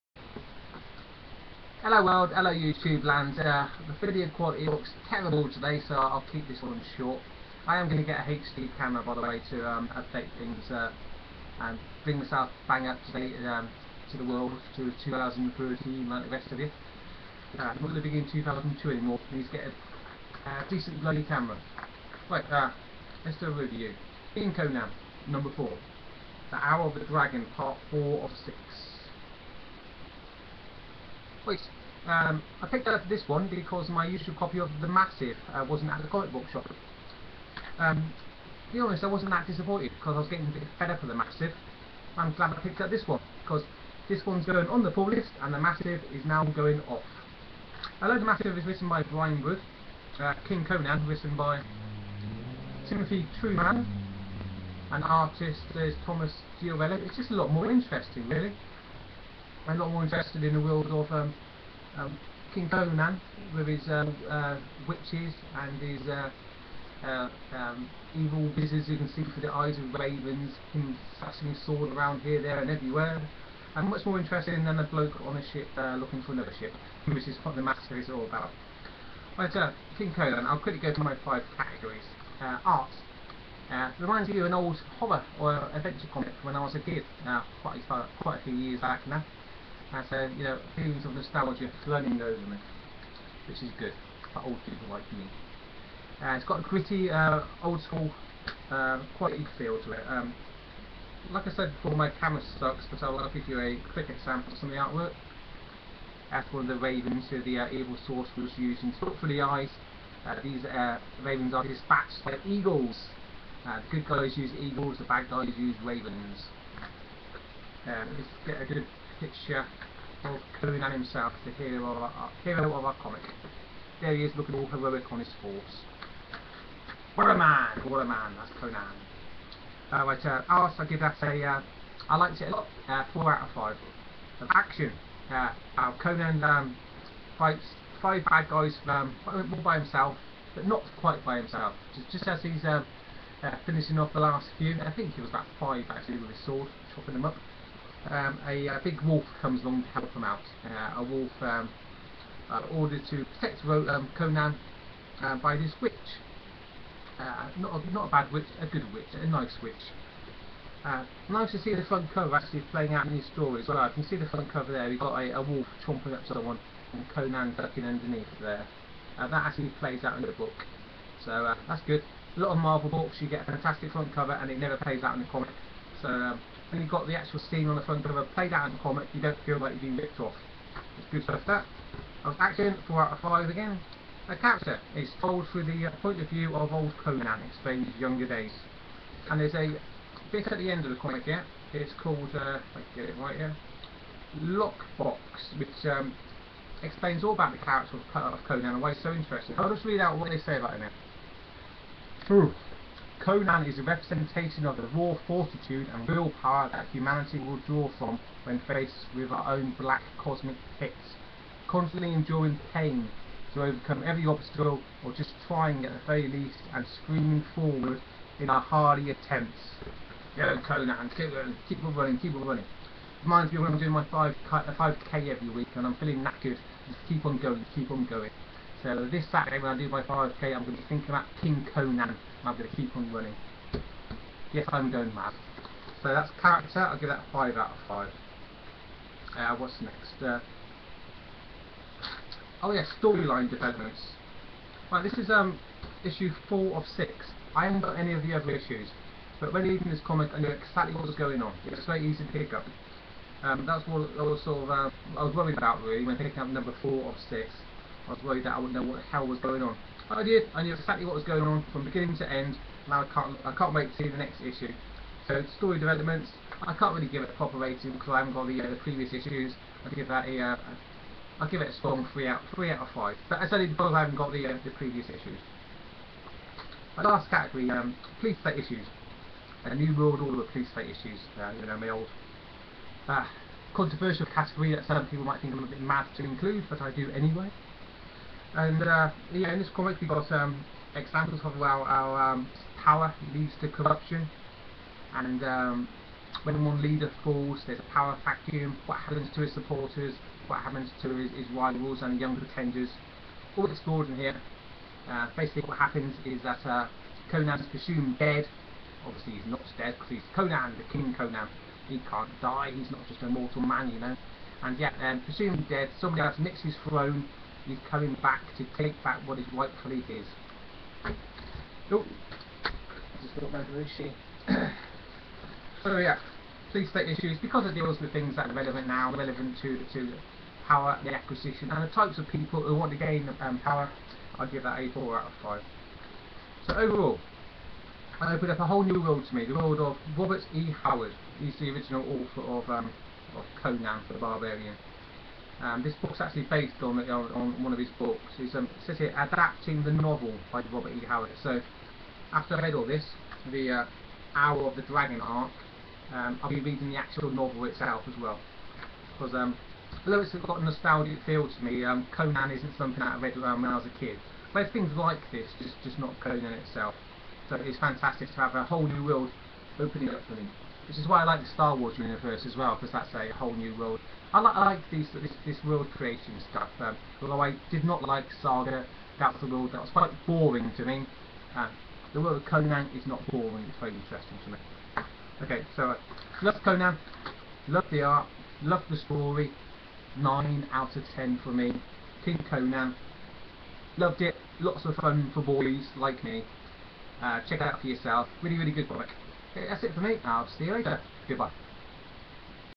KING CONAN #4: I've discovered a brand new world, and I like it.
I review King Conan #4 (The hour of the Dragon Part 4 of 6) by Timothy Truman and Tomas Giorello. Another fantastic comic from Dark Horse. Loved it. This one's now been added to my pull list. Rating: 10/10.